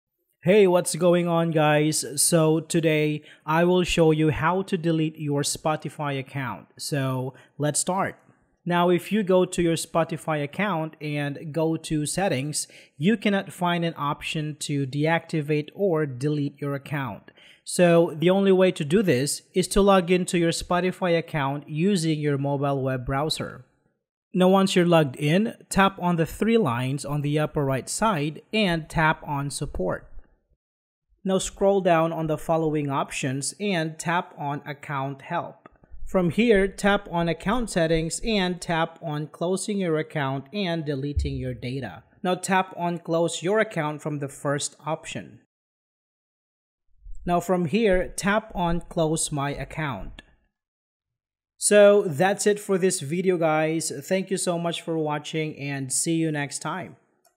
How To Delete Your Spotify Account 2024#tutorial2024 howto #2024 spotify Account Delete Tutorial#
How to Delete Your Spotify Account (2024)

Log In: Sign in to your Spotify account.

Access Support: Scroll to the bottom and click on "Support."

Account Settings: Navigate to "Account Settings."

Close Account: Click on "Close Account" and follow the prompts to confirm your decision.

Verify: Check your email for a confirmation link to permanently delete your account.

Follow these steps to delete your Spotify account in 2024.